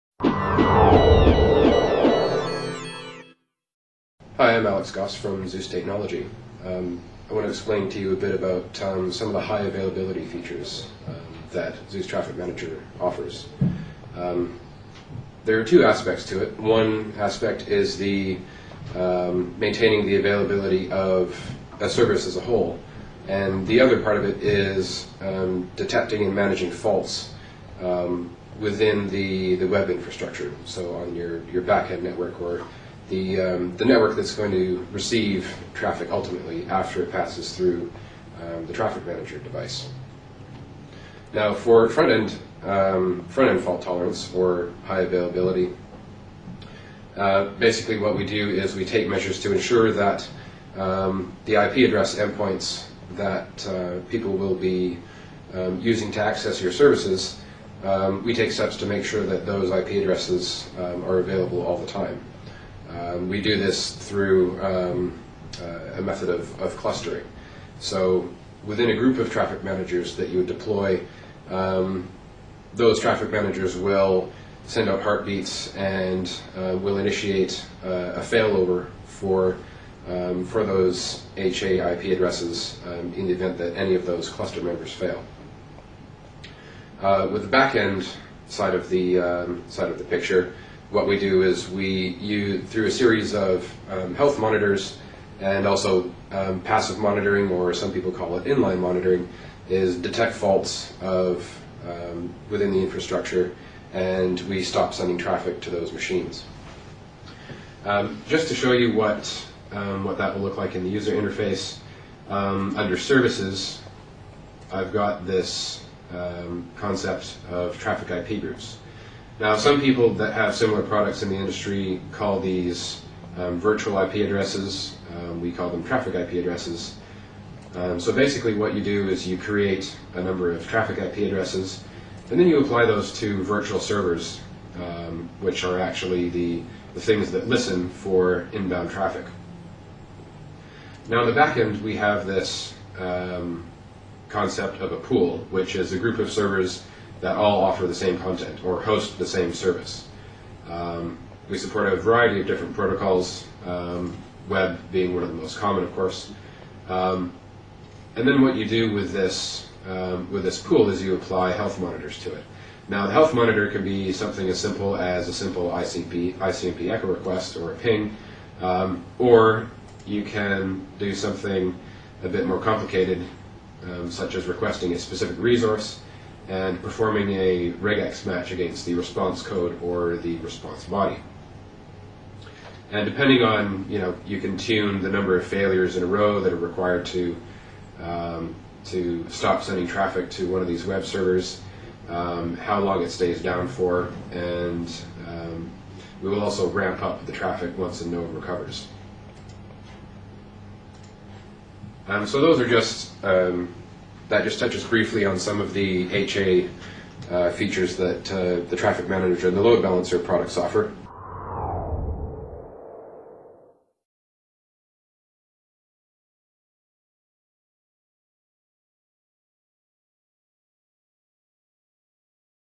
Riverbed Stingray Traffic Manager - High Availability
Stingray Developer Alex Gosse discusses some of the High Availability features found in Stingray Traffic Manager.